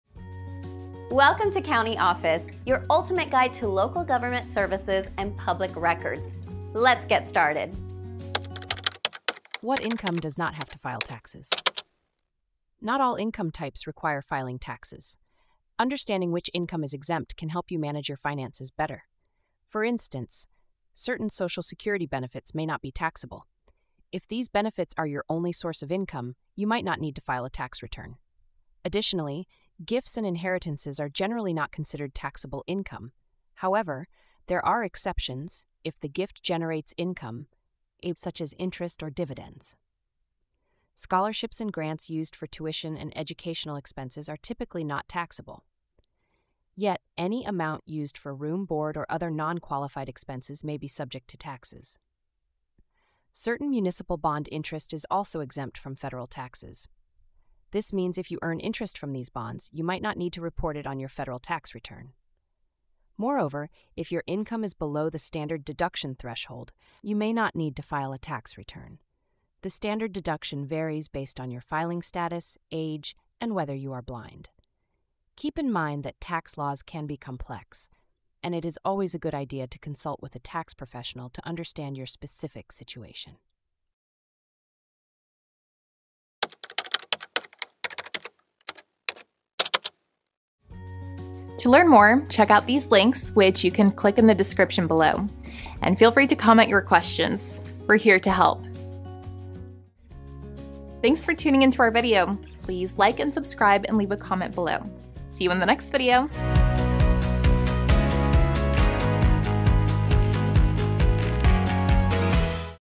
What Income Does Not Have To File Taxes? - CountyOffice.org
What Income Does Not Have To File Taxes? Have you ever wondered which income types do not require filing taxes? In this enlightening video, we delve into the various sources of income that may be exempt from taxation. From social security benefits to gifts and inheritances, we explore the nuances of taxable and non-taxable income. Scholarships, grants, and municipal bond interest are also discussed in detail, shedding light on what you may or may not need to report on your tax return. Understanding these exemptions can significantly impact your financial planning. Remember, tax laws are intricate, so consulting a tax professional is advisable for personalized guidance.